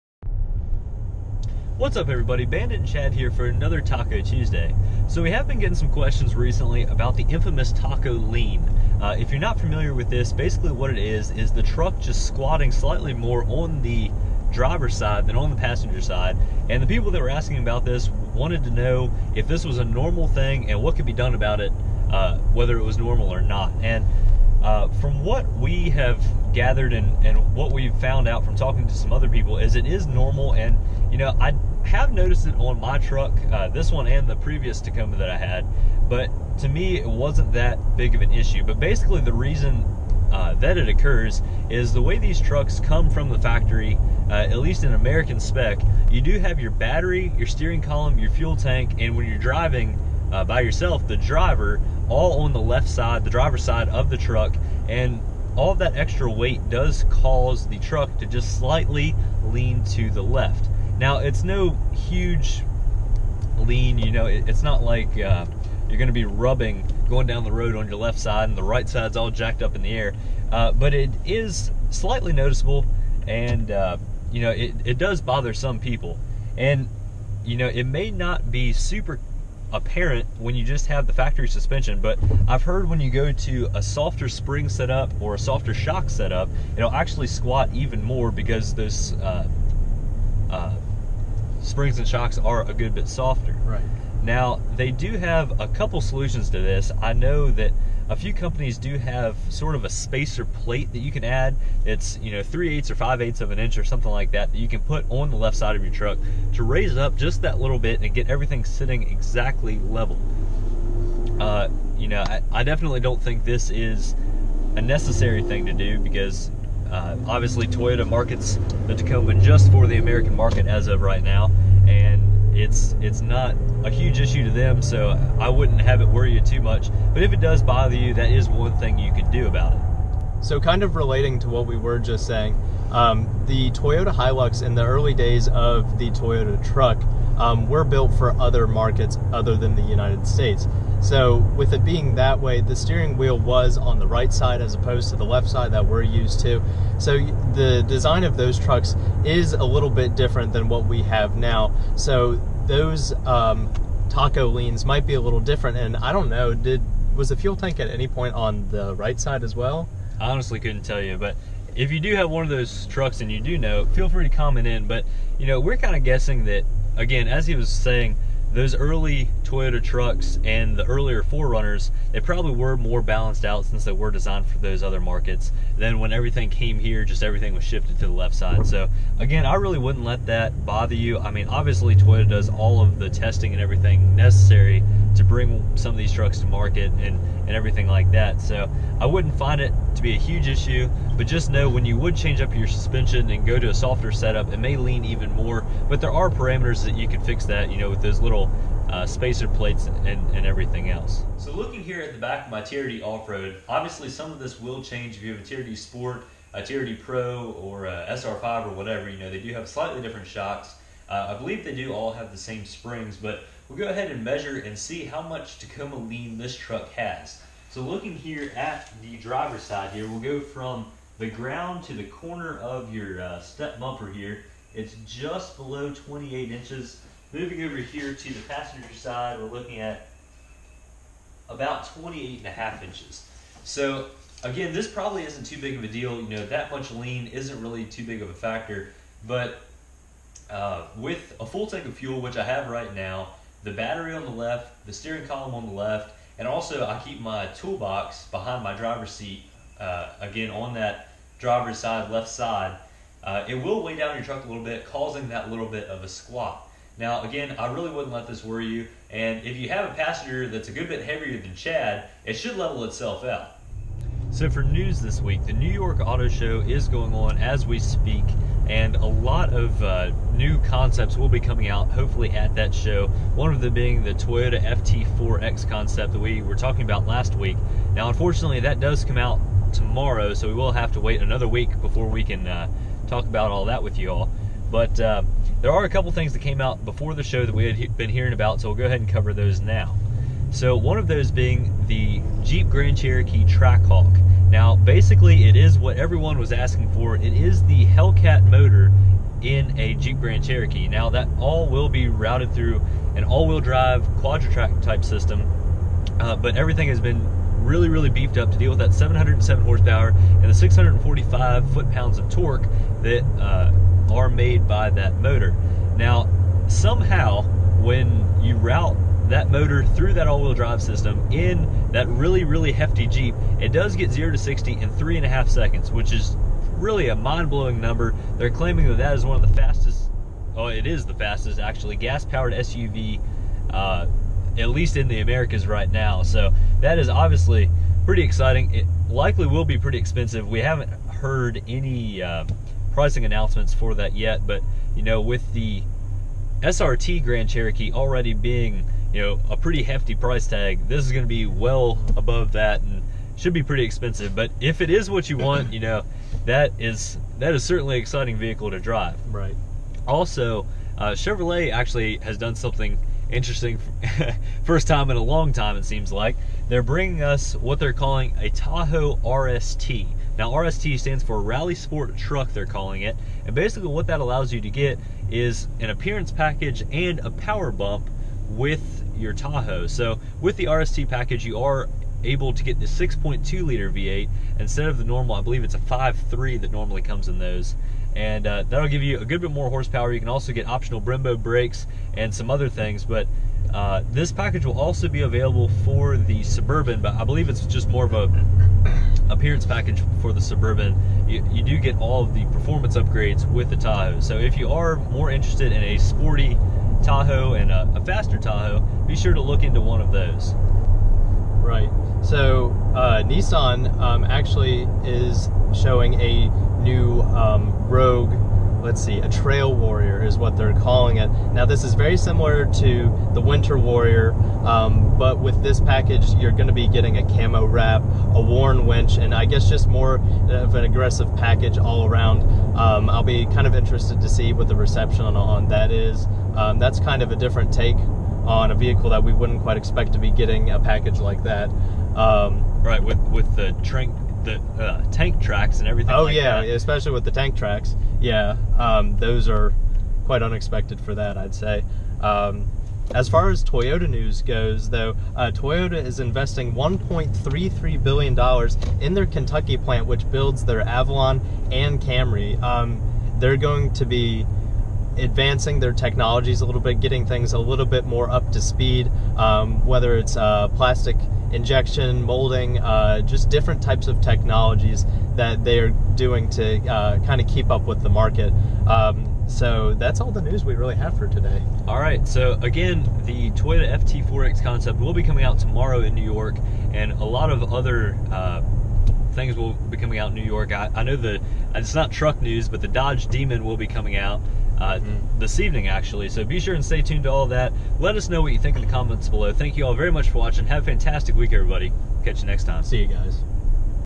2016 Tacoma Weekly Update #54: Do YOU have Tacoma Lean??? We Explain & This Week's News!
Have you ever wondered why your Tacoma (or any truck) leans more to the driver's side? We explain why! Not only do you sit on that side of the truck everyday, but your when your gas tank is full that is a substantial amount of weight on just one side of the truck. Plus your suspension could also have something to do with this and it isn't anything to really worry about! In addition to that, we talk about this week's news!